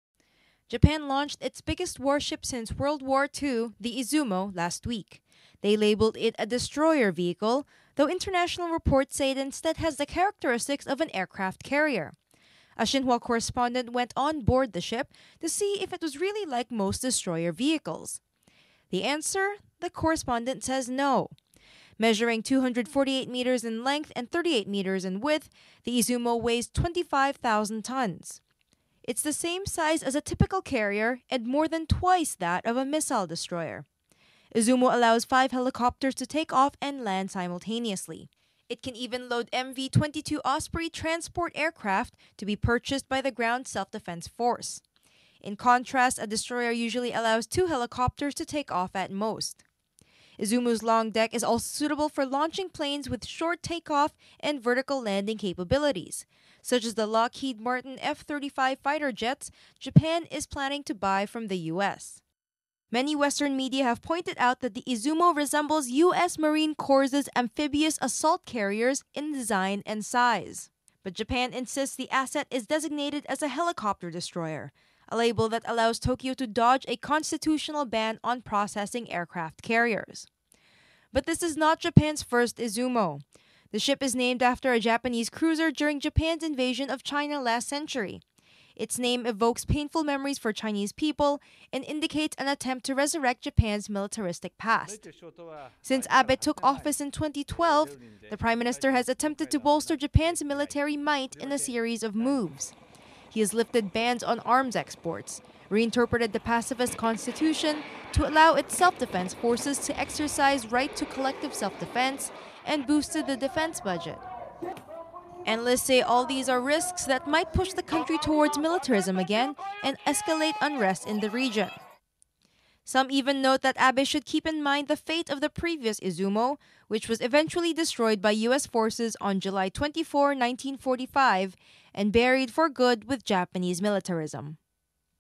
Correspondent went on board "Izumo", a destroyer or carrier?
Japan launched its biggest warship since World War II, the Izumo, last week. Tokyo labelled it a Destroyer vehicle, though international reports say it instead has almost all the characteristics of an aircraft carrier.  So which is right? And why?